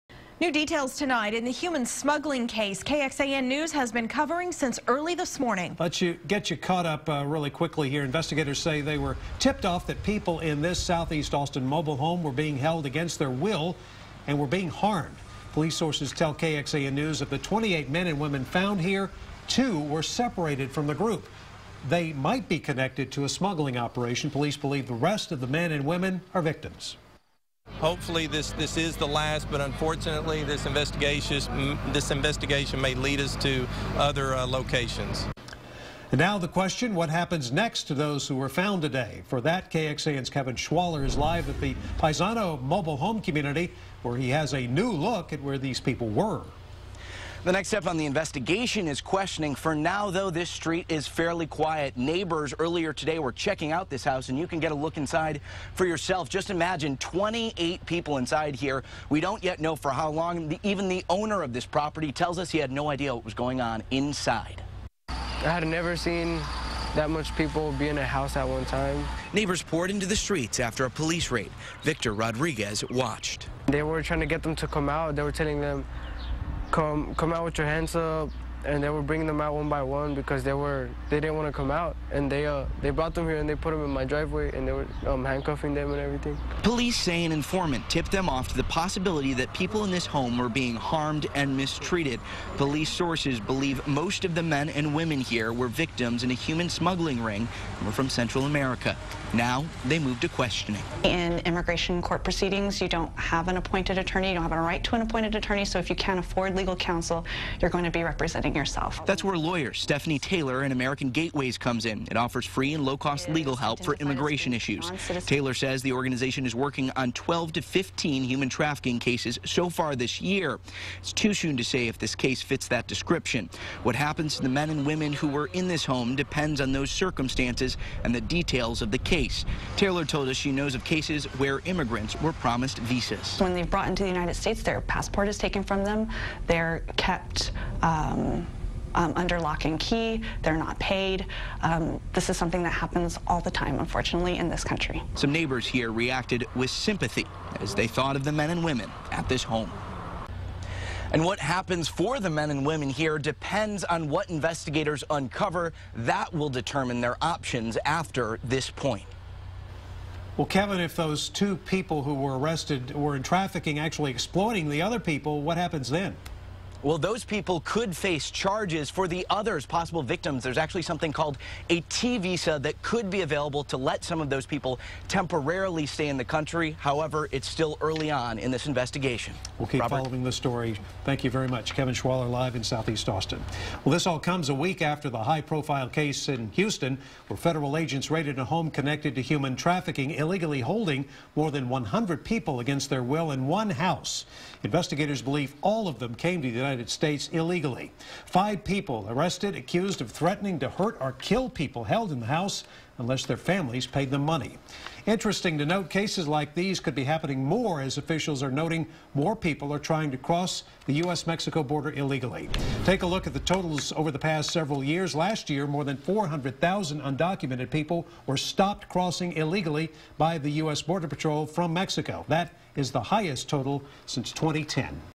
Police raid busts human smuggling ring in Southeast Austin
After hours of staking out and waiting, Austin police busted one of the largest human smuggling operations in recent years, detaining 28 people inside the home.